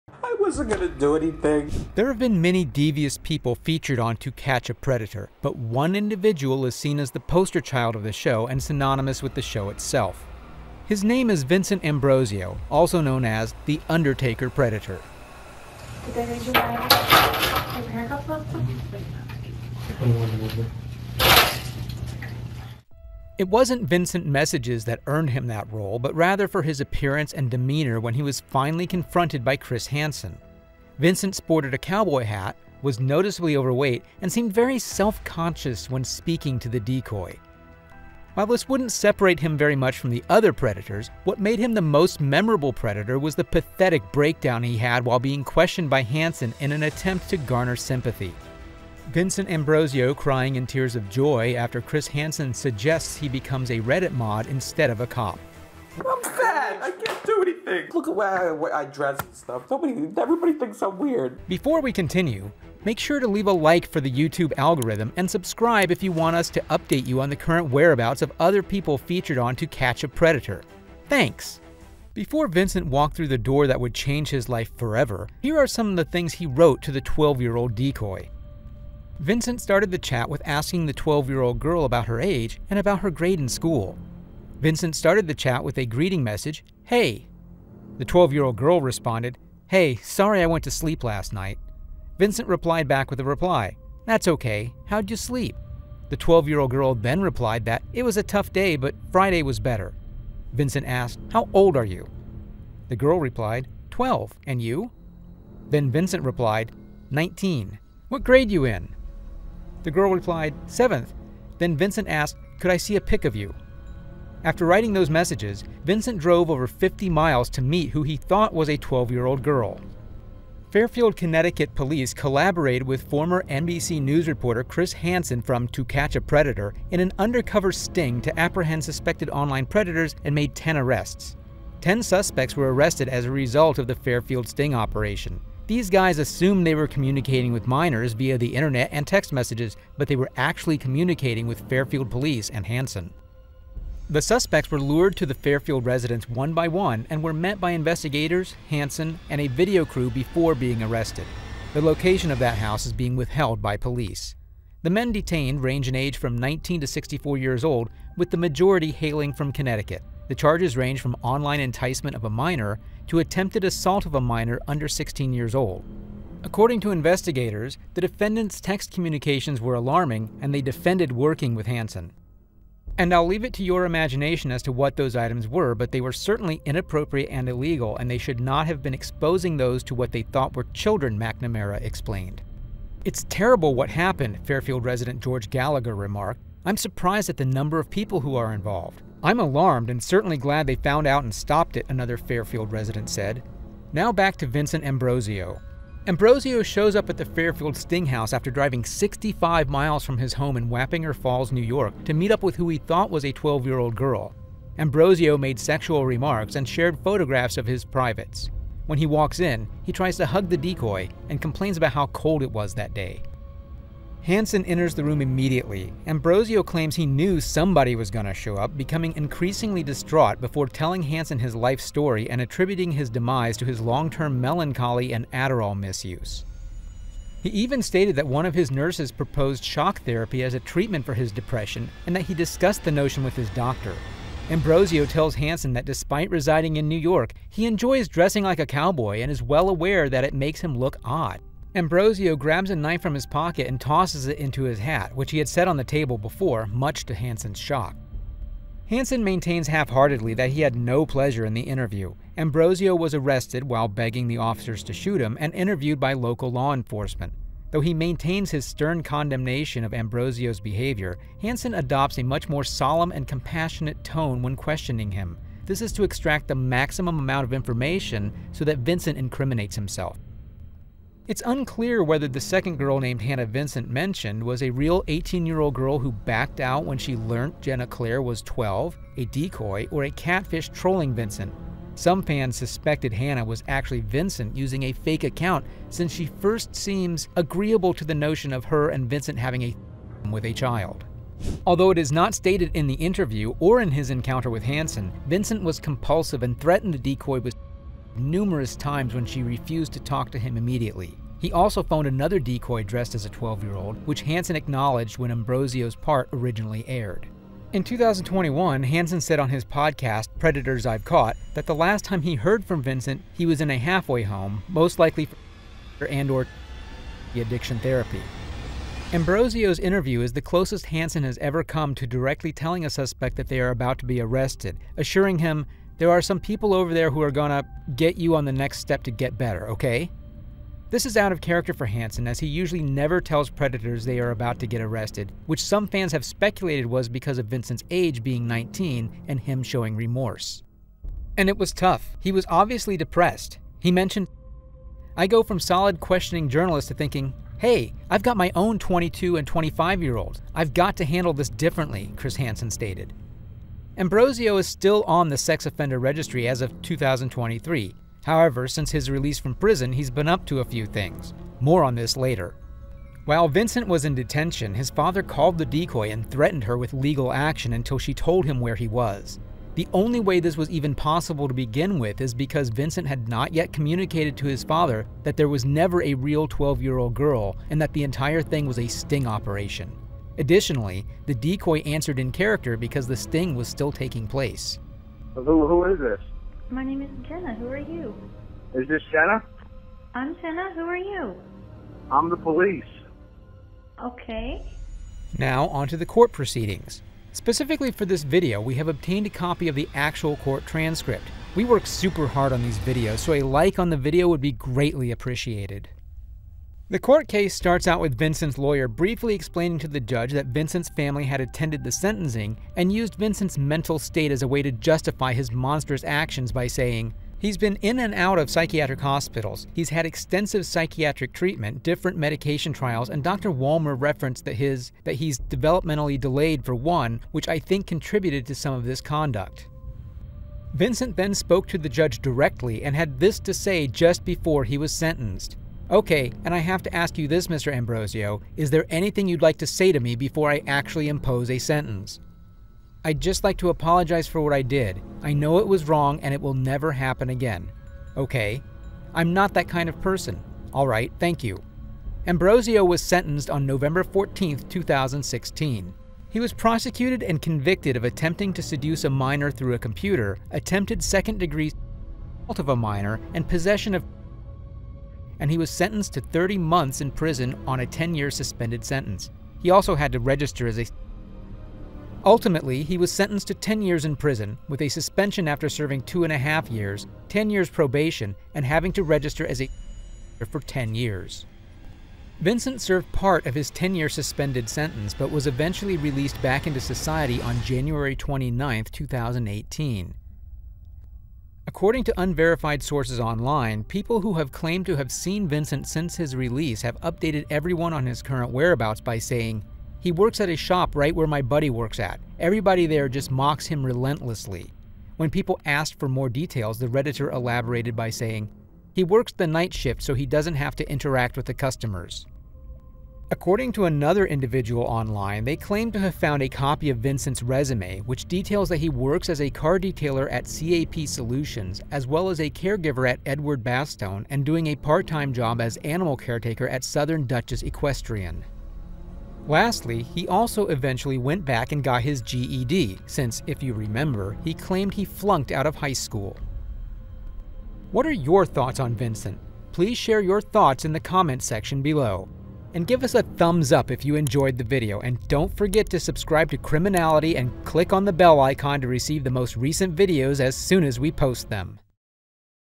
What Happened to the Undertaker Predator? (Vincent Ambrosio)
On Chris Hansen's famous show To Catch a Predator and Hansen Vs Predator Vincent Ambrosio also known as The Undertaker Predator was one of the more memorable people featured on TCAP. Here's where he's at in 2023.

This video was inspired by SunnyV2 Film Digest Generic White Youtuber